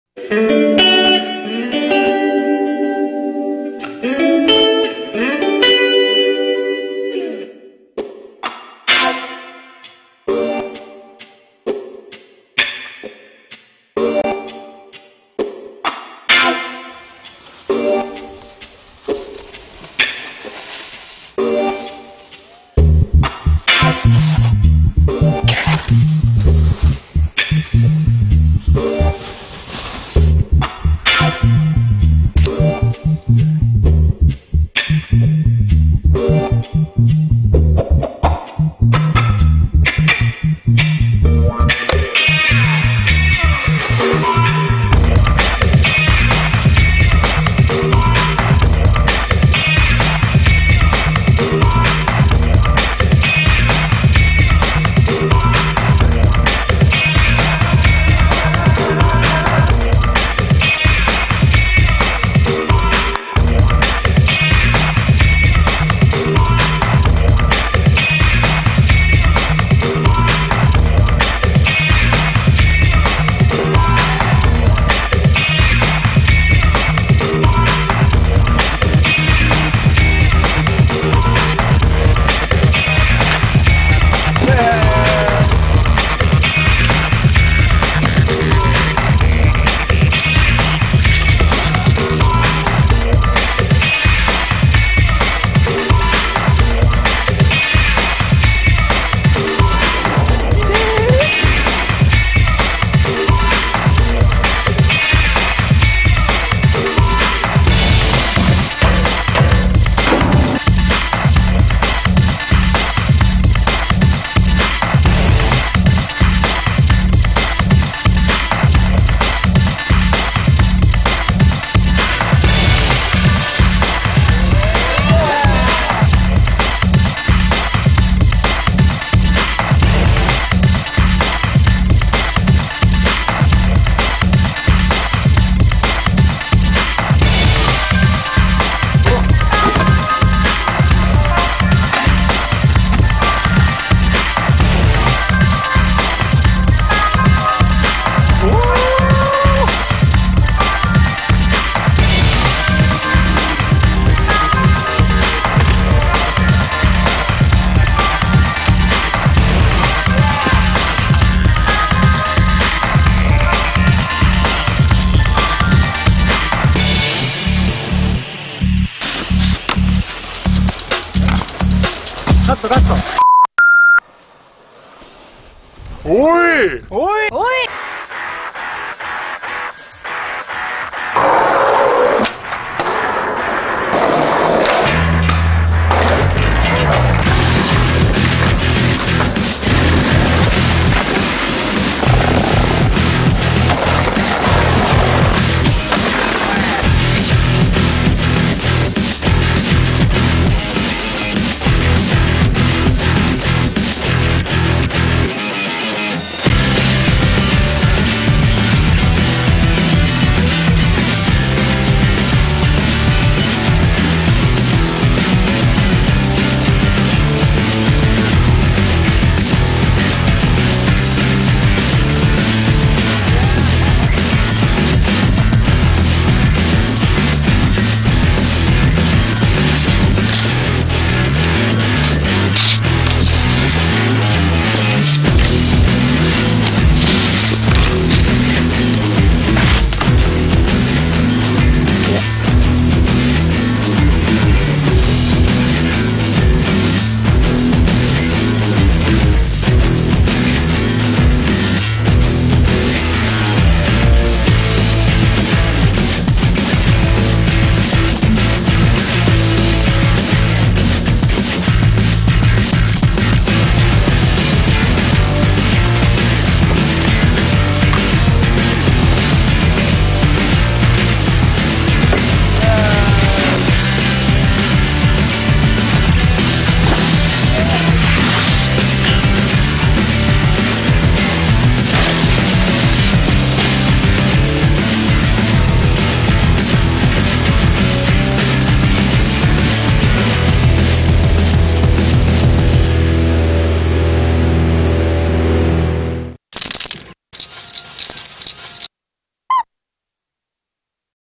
GUCCIES
長野県白馬エリアで活動するARON FILM制作のスノーボードムービー。メトログラスのエックスサイドへの投稿映像。